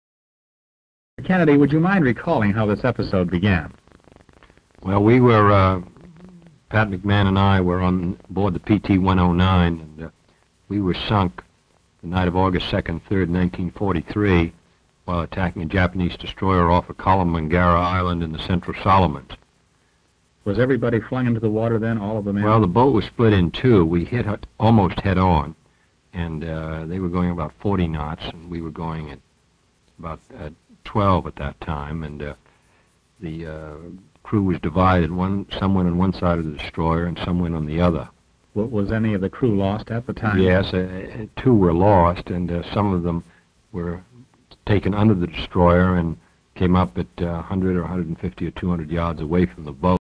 John F. Kennedy visits San Diego in 1957, shares story of WWII battle
Senator John F. Kennedy talked about saving the life of a shipmate during WWII by towing him to an island after the PT boat was sunk by a Japanese destroyer.

August 13, 1957 Original script:
John F. Kennedy, Democratic Senator from Massachusetts, was in San Diego today along with Congressman Torbert MacDonald to act as technical supervisor in the filming of a television film on San Diego bay.
Kennedy is the man that many Washington insiders pick as the democratic candidate for President in 1960 or 1964.
The television story being filmed relates an incident in Kennedy's naval service during World War II in which Kennedy saved the life of a shipmate by towing him to an island after the PT boat was sunk by a Japanese destroyer.
Harold Keen asked Senator Kennedy to describe the incident and in this film he recalls the harrowing ordeal from August 2-3, 1943.
He mentions Pat McMahon in the interview, the man he saved. He was an Encinitas resident from 1975 until his death in 1990 at the age of 84. They remained friends until President Kennedy's death in 1963.  According to IMDB.com, the television show aired on October 23, 1957 and John Baer portrayed John F. Kennedy.